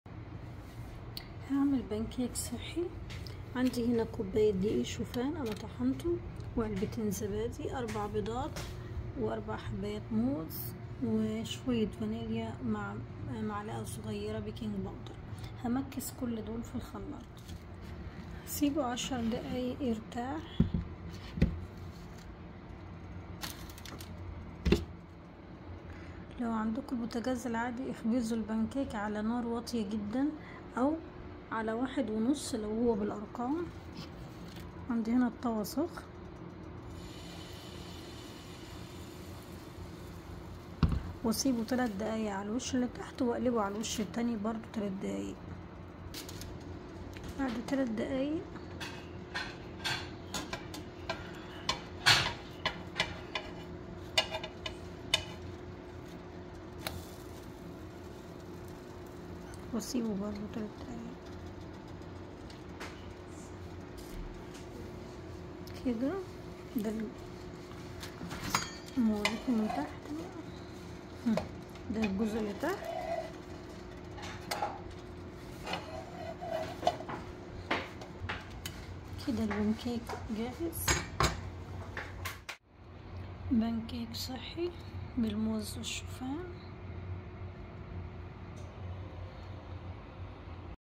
صنف (235) 🥞بان كيك صحي بالشوفان والموز👍
المكونات :
الكمية دي بتعمل (12_ 15) قطعة متوسطة الحجم
علبة زبادي قليل الدسم
حبيتين 🍌
حبيتين 🥚
نص كوب شوفان مطحون
نص م ص بيكنج باودر
ظرف فانيلا
لما ضاعفت الكمية عملت المقادير دي :
كوب شوفان مطحون
3 🍌
3 🥚
1 م ص بيكنج باودر
علبة زبادي
ظرفين فانيلا